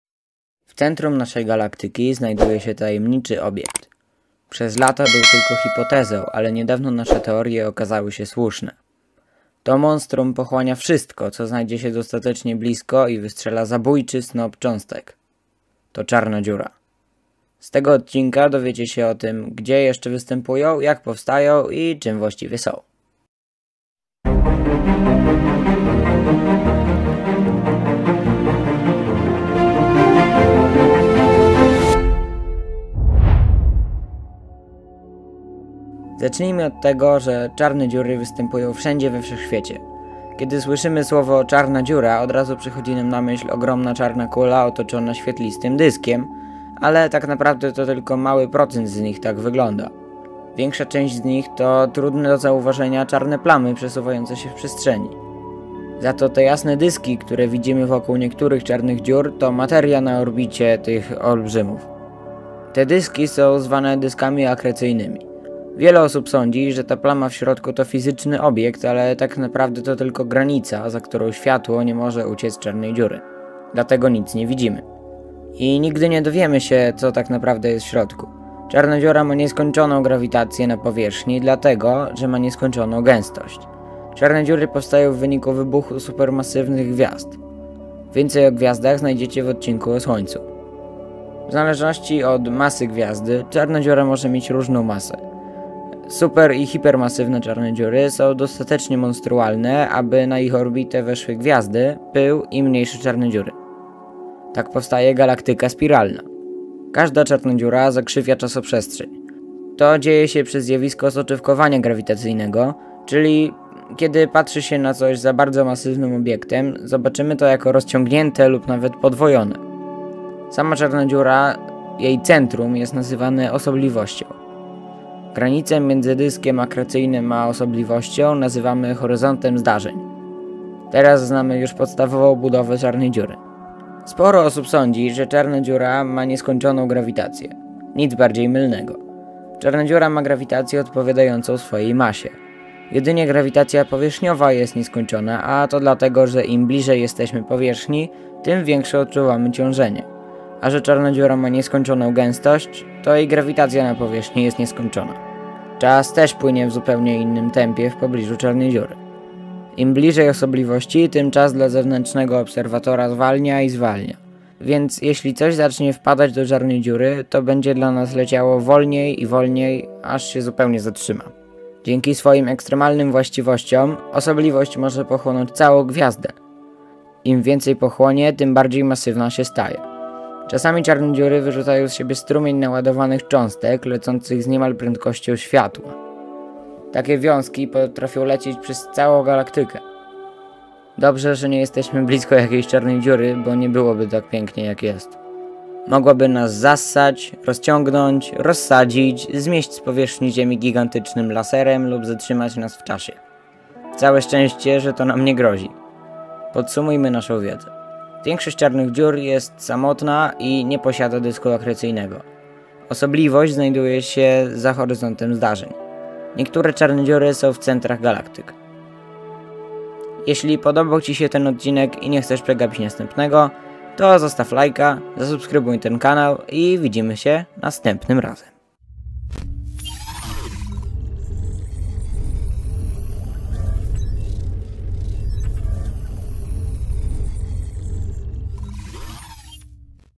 Czym tak naprawdę są CZARNE DZIURY?
Z tego odcinka dowiecie się o jednych z najdziwniejszych obiektów we Wszechświecie - czarnych dziurach.

MEDIA